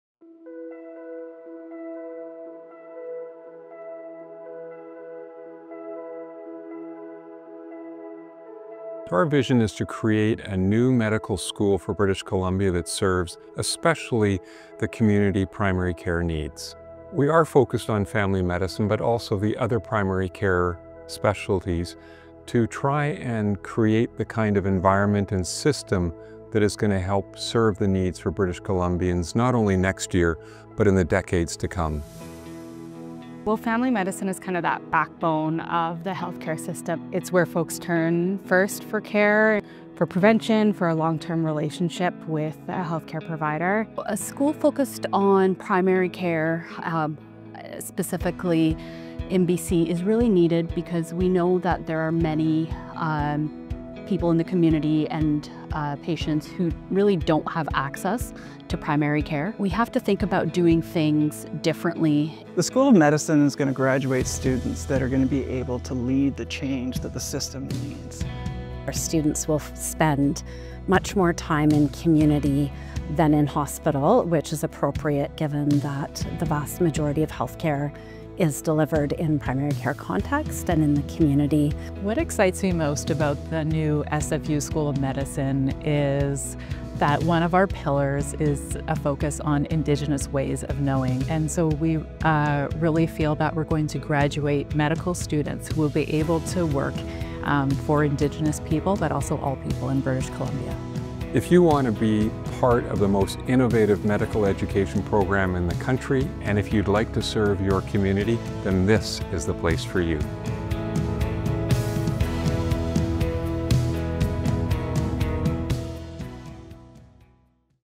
Welcome to the SFU School of Medicine
SFU School of Medicine - Welcome Video

The SFU School of Medicine is the first new medical school to be built in western Canada in almost 60 years. We are preparing a new generation of skilled physicians who reflect the communities they serve, with the clinical expertise and collaborative abilities to be outstanding primary care physicians.